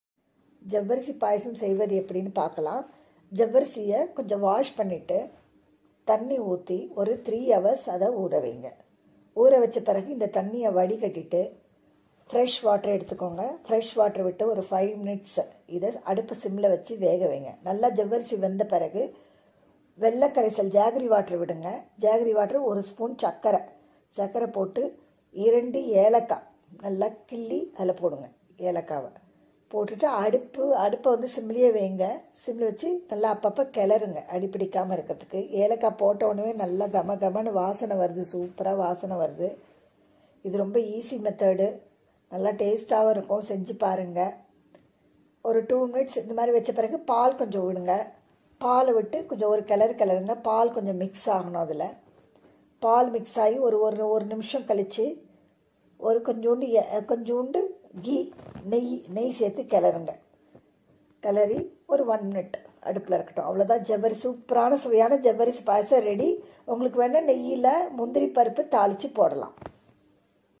ஜவ்வரிசி பாயாசம் | Sago Kheer | Easy to cook
ஜவ்வரிசி ( Sago)
வெல்லம் ( jaggery)
சர்க்கரை ( sugar)
பால் ( Milk)
நெய் (Ghee)
முந்திரி திராட்சை தேவைபட்டால்
Cashew nuts and rasins (optional)